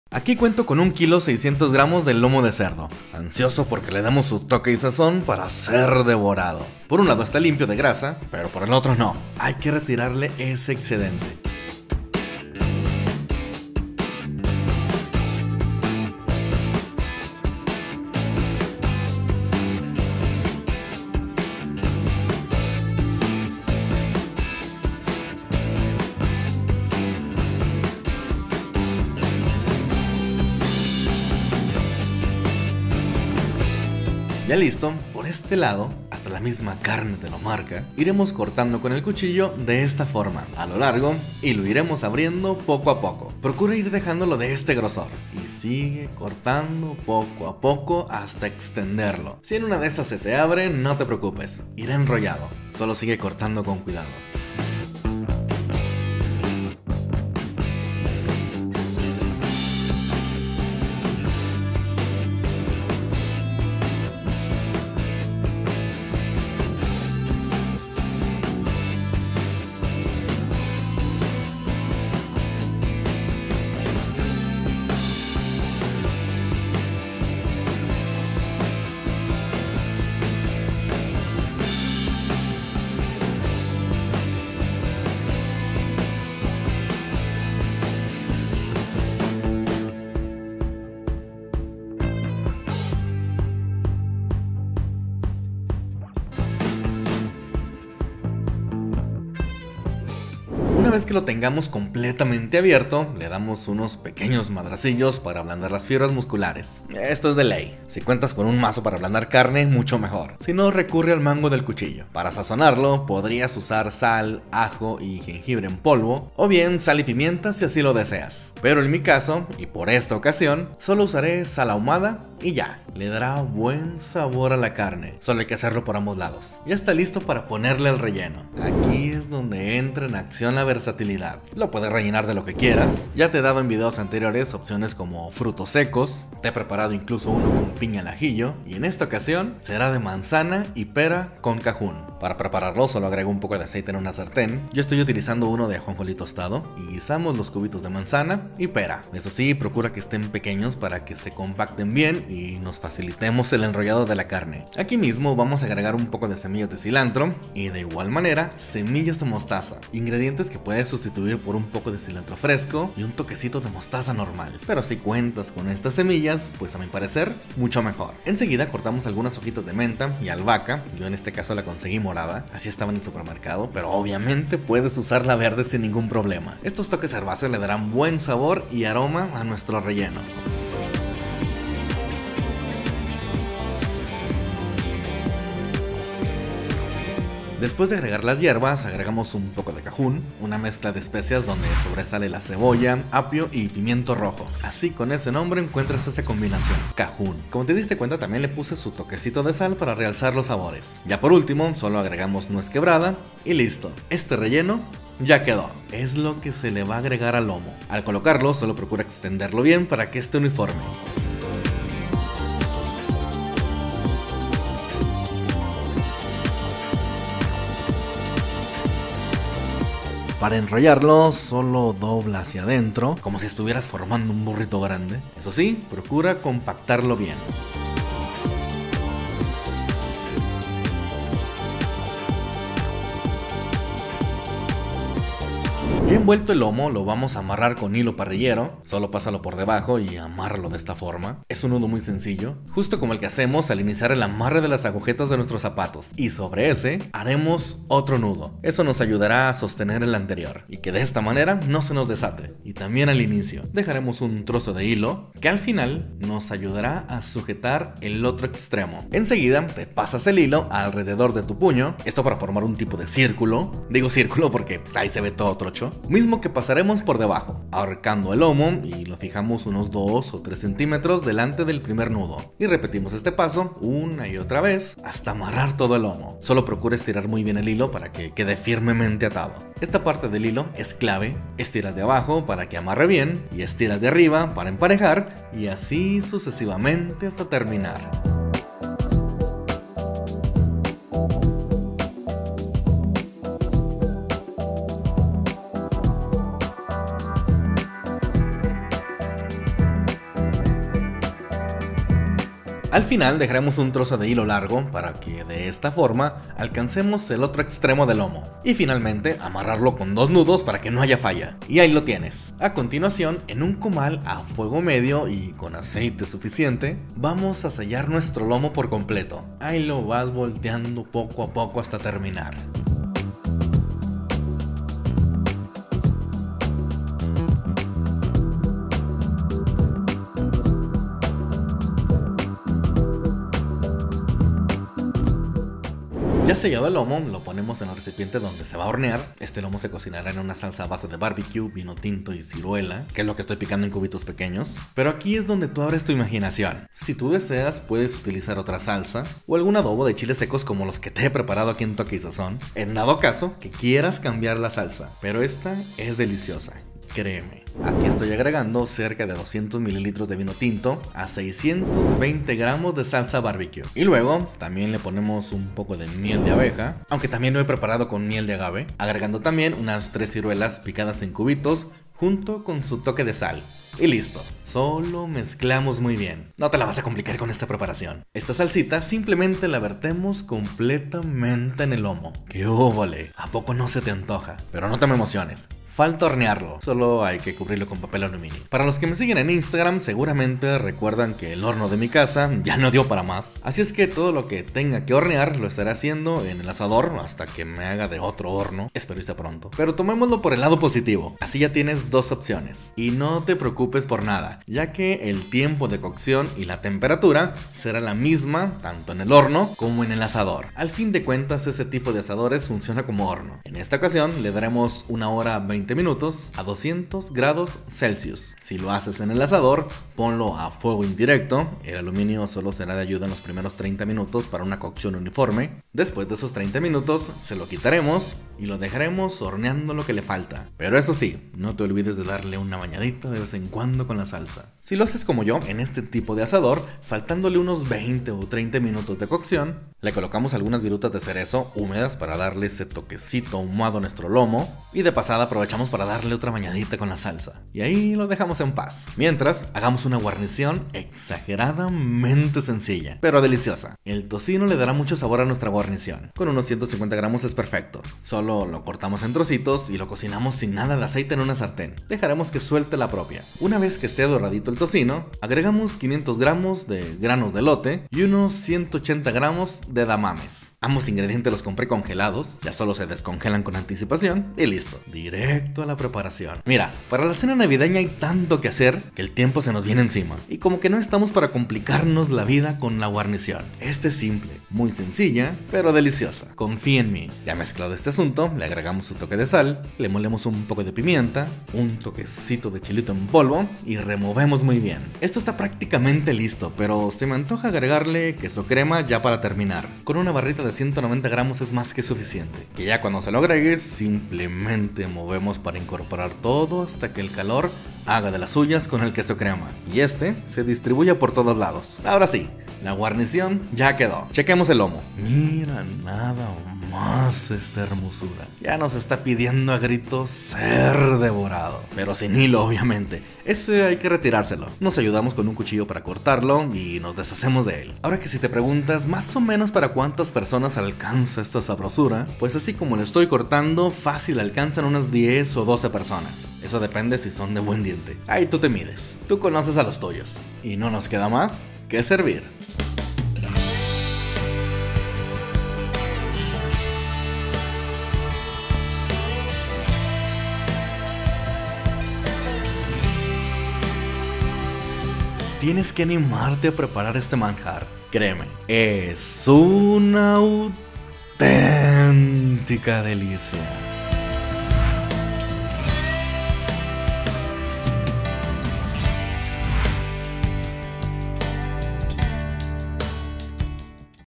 LOMO en SALSA BBQ Relleno de Manzana Cajún | TOQUE Y SAZON
Un Lomo de cerdo relleno de manzana y pera con cajún bañado en salsa BBQ Ideal para una cena familiar espectacular!